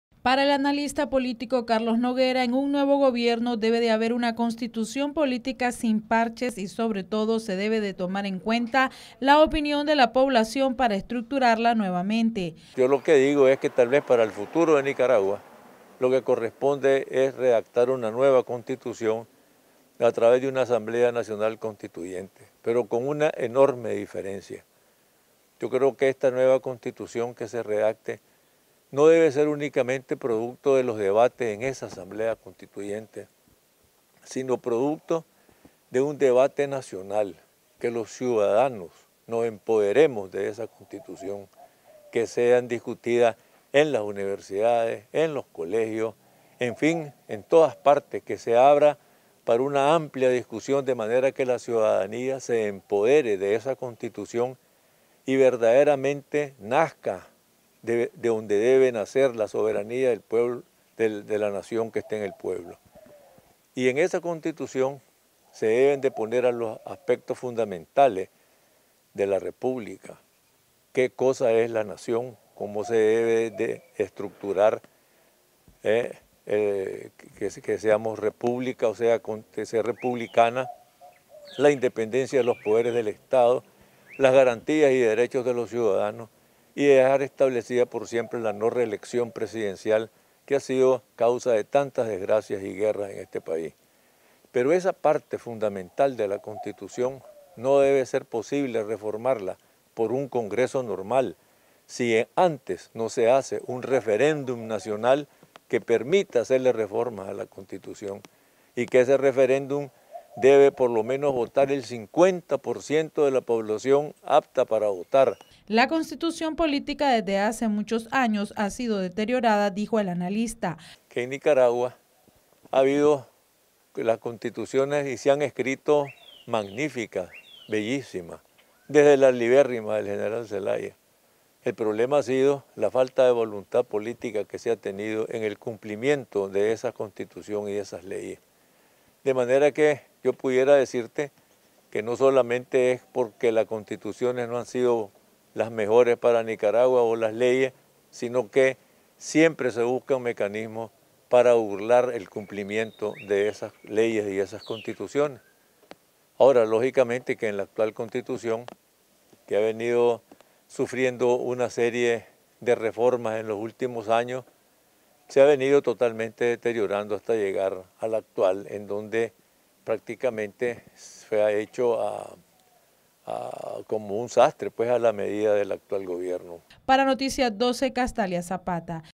Con nuevo gobierno se debe crear una nueva constitución
NosResistimosAlEmbargo En un gobierno democrático debe crearse una nueva constitución política, dijo el analista político Carlos Noguera